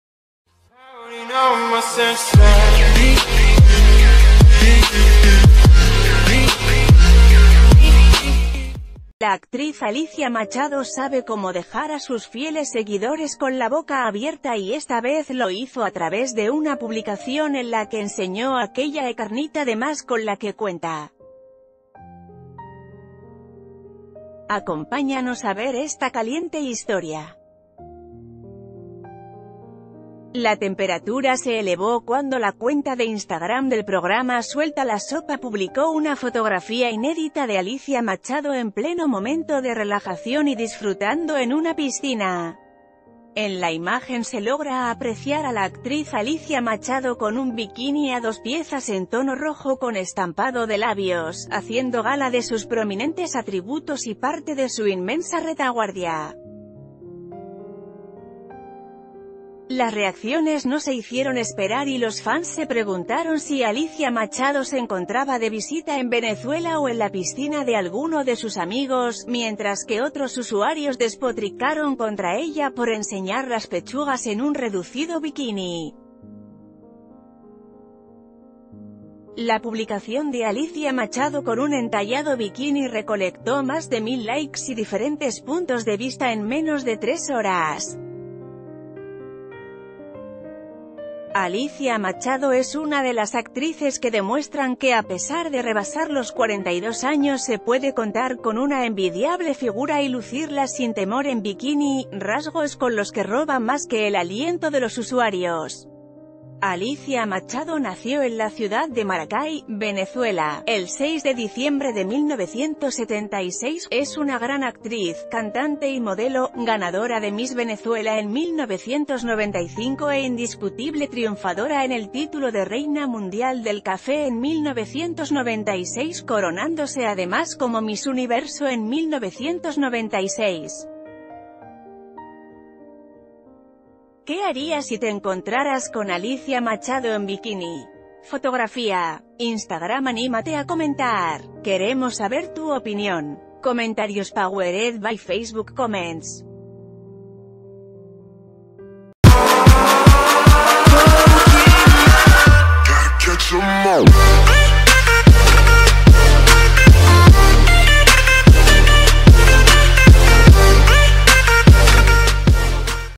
Destrozan a Alicia Machado por su físico tras presumir pechos en bikini
La actriz Alicia Machado sabe cómo dejar a sus fieles seguidores con la boca abierta y esta vez lo hizo a través de una publicación en la que enseñó aquella ‘carnita de más’ con la que cuenta. Acompáñanos a ver esta caliente historia. La temperatura se elevó cuando la cuenta de Instagram del program...